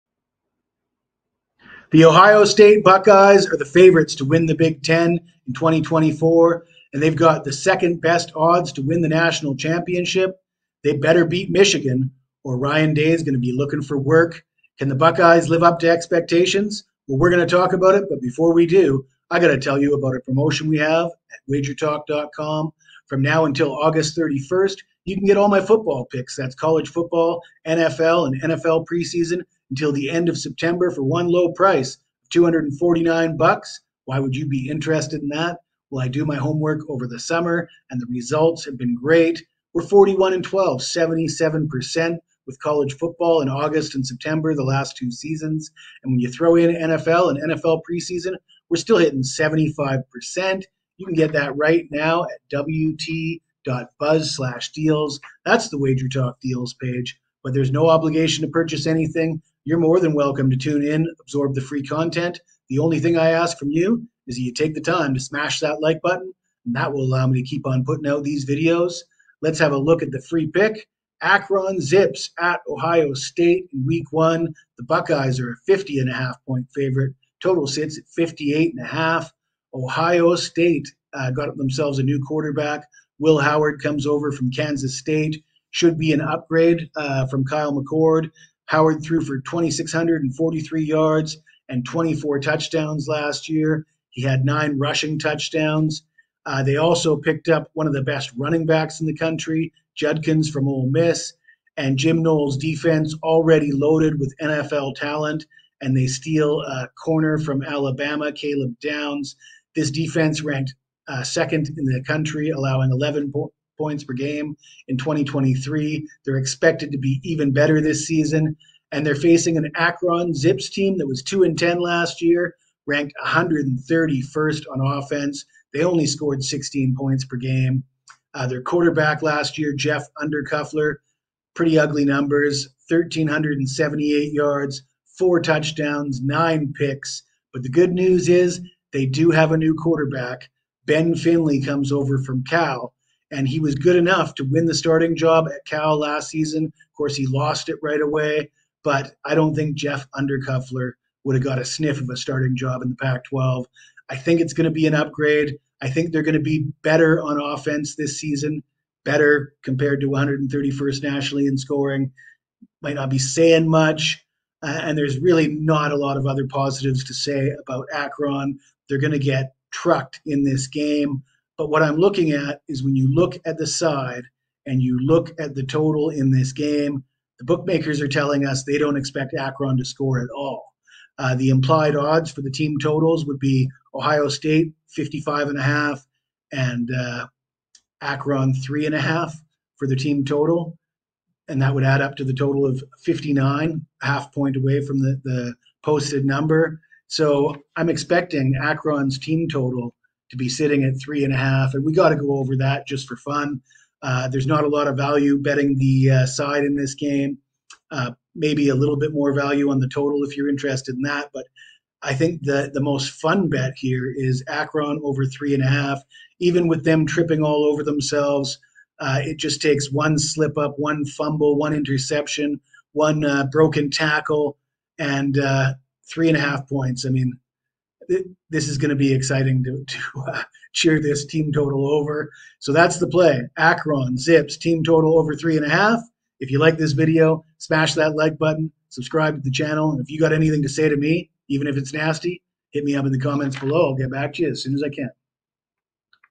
Akron vs Ohio State Prediction and Picks | 2024 College Football Week 1 Best Bets
If you are looking for 2024 College Football Week 1 picks and predictions for the Akron vs Ohio State match-up, we have you covered! Our college football handicapping experts discuss this matchup from a sports betting perspective, sharing their picks and predictions vs. the current odds.

🏈 Akron vs Ohio State College Football Week 1 Betting Preview 🏈

🕒 Time: 3:30PM ET / 12:30PM PT
📍 Location: Ohio Stadium in Columbus, OH
📺 TV Broadcast: CBS

📱 Team Hashtags:

Akron Zips
Ohio State Buckeyes

📊 Current Spread: Ohio State -50.5
📈 Total: 58.5

🔍 Matchup Overview: The Akron Zips travel to Columbus to face in-state rival Ohio State Buckeyes in a thrilling college football season opener. Both teams are eager to start the season strong with a statement win.

🌟 Interesting Facts:

Akron Zips: The Zips are looking to bounce back from a disappointing 2023-24 season which ended with a 2-10 record. The last time the Zips faced Ohio State they lost 59-7 in 2021.

Ohio State Buckeyes: A powerhouse in college football, Ohio State boasts numerous conference championships, but are looking to get back to the Big Ten Championship Game having not made it now since 2020.

Tune in for an exciting game as the Zips and Buckeyes clash in what promises to be an unforgettable matchup!